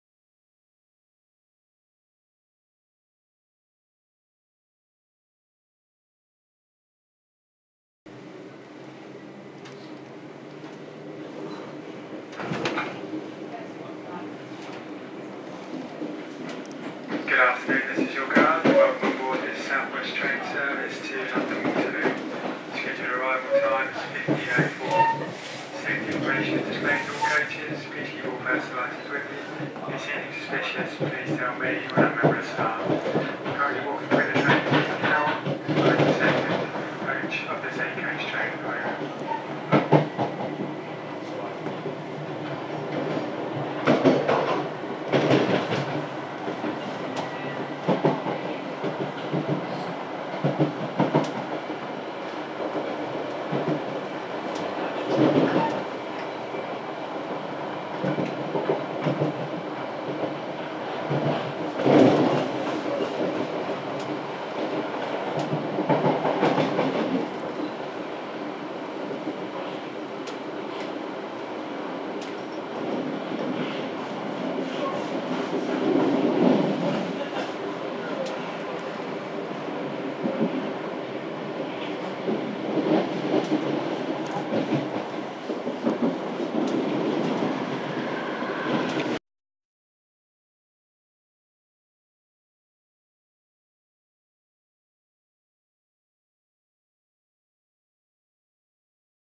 Leaving Wimbledon
On board a SWT service leaving Wimbledon